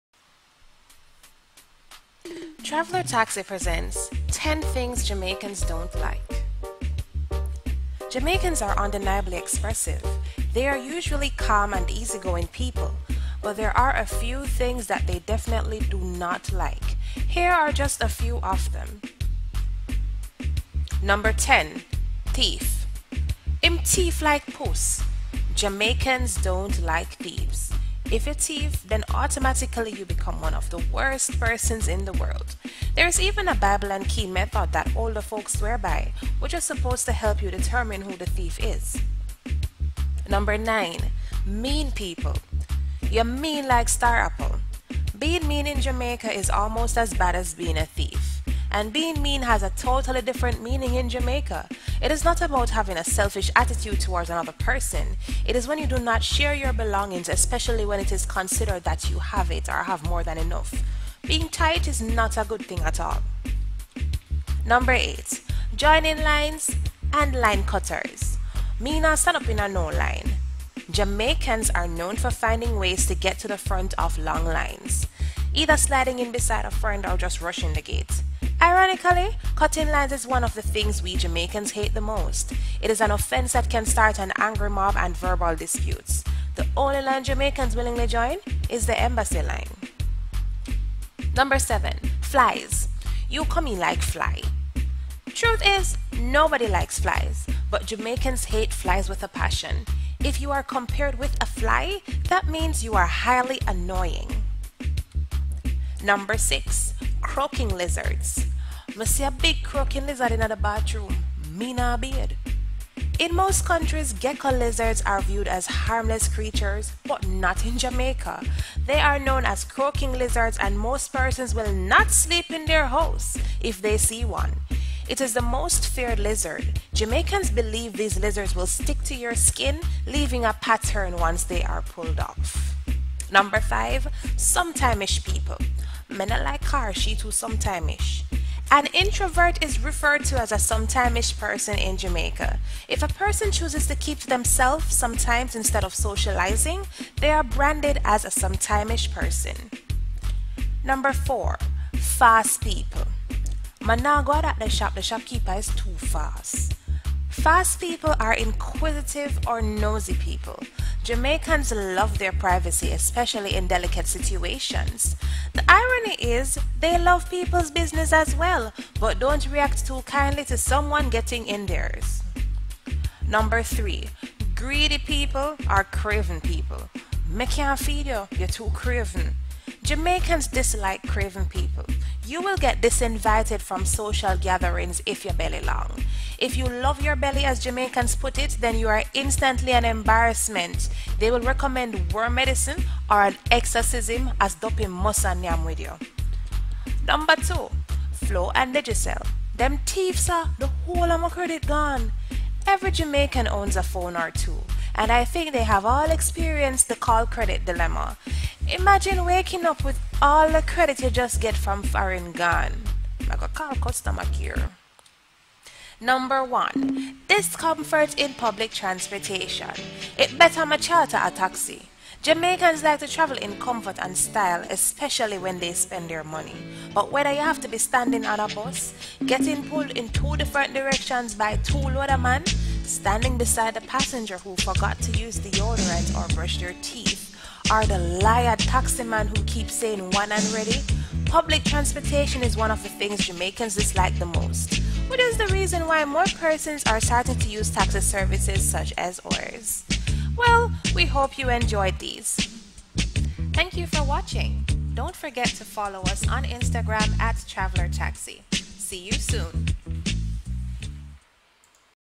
10 Things Jamaicans dislike
This video lends a bit of humor to 10  things that Jamaicans dislike.